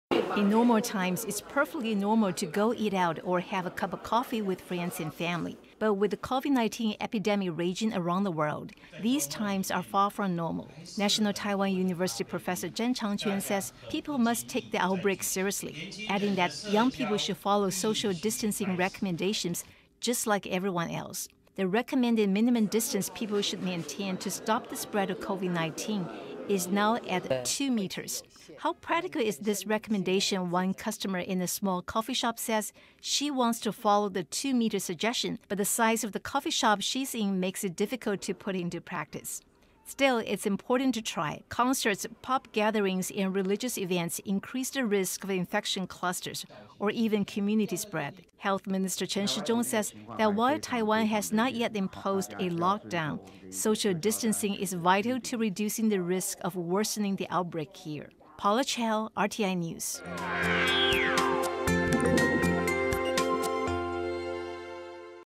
People should keep two meters apart to prevent COVID-19: Expert | Taiwan News | RTI
National Taiwan University public health professor Chan Chang-chuan say people should stay at least two meters apart from one another to prevent COVID-19 from spreading.

Chan’s comments come amid a surge in the number of Taiwan’s confirmed COVID-19 cases, particularly cases imported from abroad.